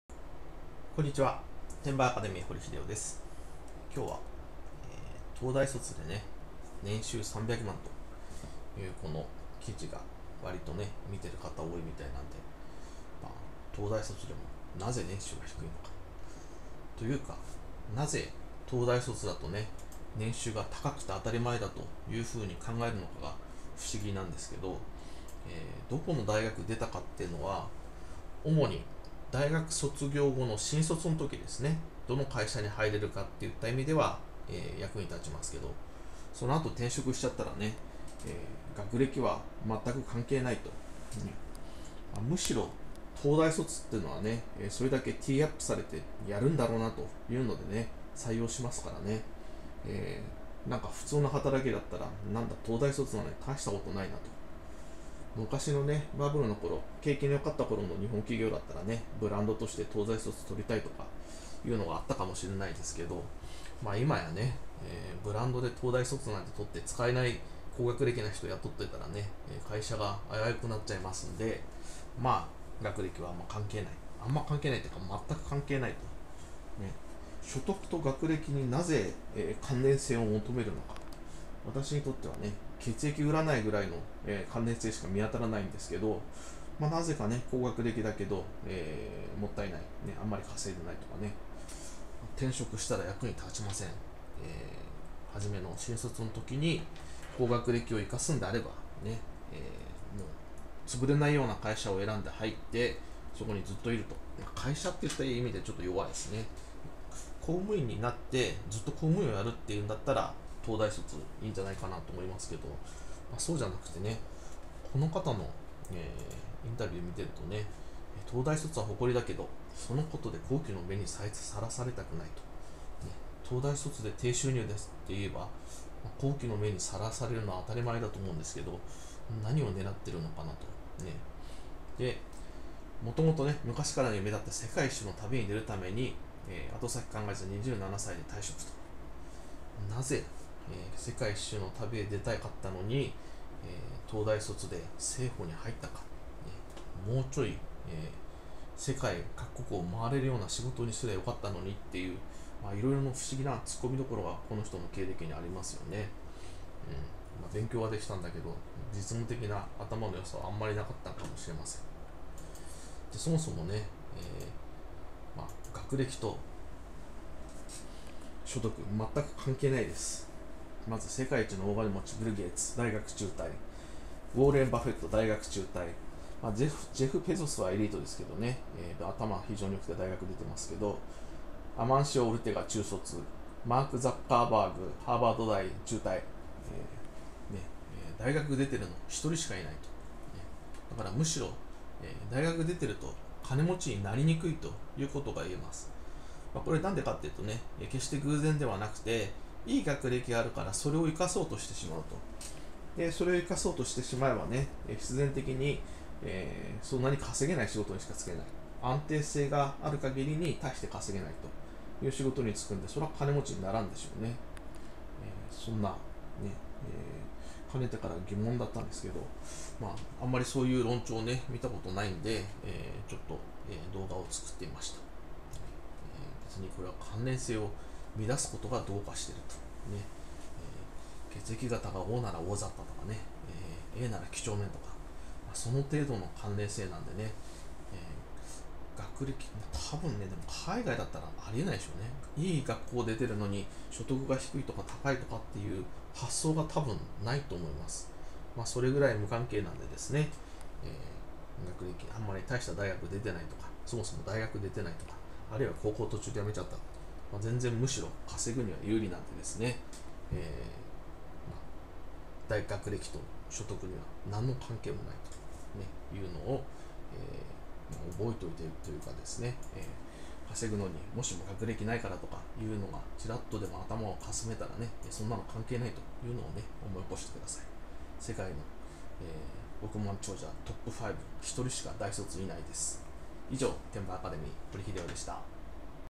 学歴は入社試験！東大卒では、まともな転職はできない
東大卒＋海外現地採用＋自由な放浪旅、この組み合わせは、とても良くない。
転職して自由に生きるなら物を売るスキルが無いと
←”無料アマゾン輸出メ－ルセミナー”メルマガ登録して下さい。

1日10分で月10万円を稼ぐ! Amazon輸出ビジネスがわかる本　出版社:ソシム (2013/11/22)